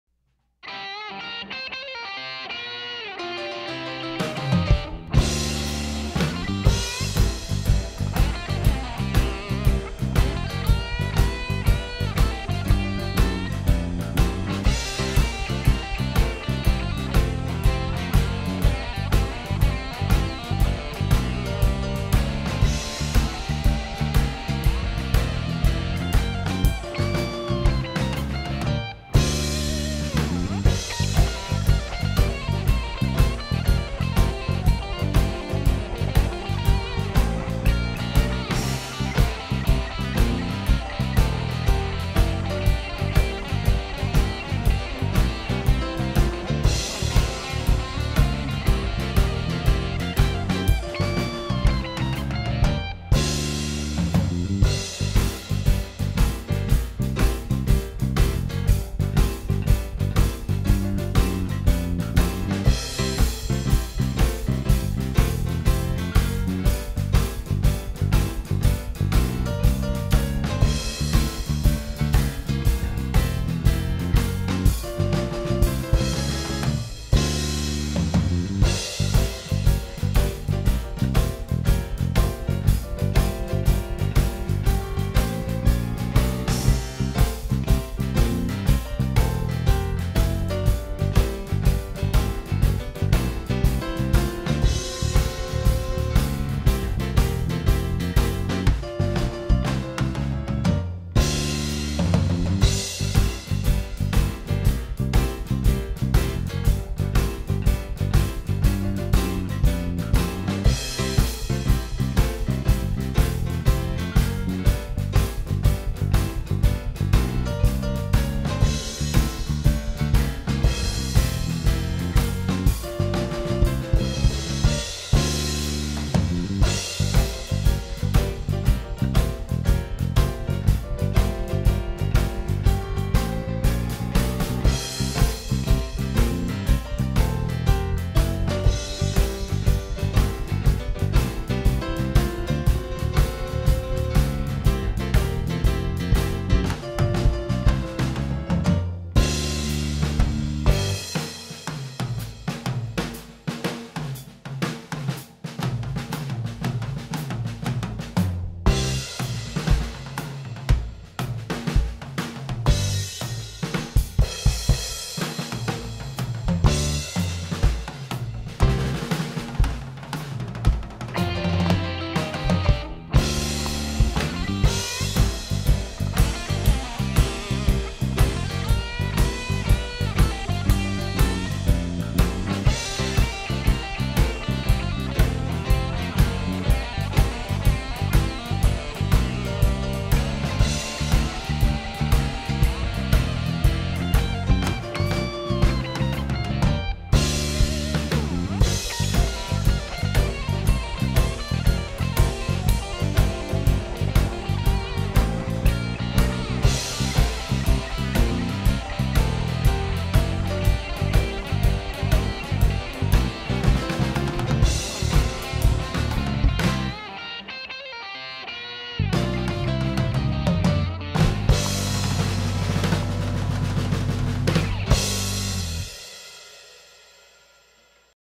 Groove 70
Bought a replacement 2002 crash finally, which is great timing for a rocking Texas Shuffle!  Also brought out my stock snare out of retirement, put some new heads on it, and tuned it to be a fat rock snare, which fits with this song.  Thinking I'll swap out this one for my brass one depending on the tune to spice up the sounds.  Also raised some of my crash stands so I'm at a better angle so I don't crack cymbals so much in the future.

Also I really need a haircut.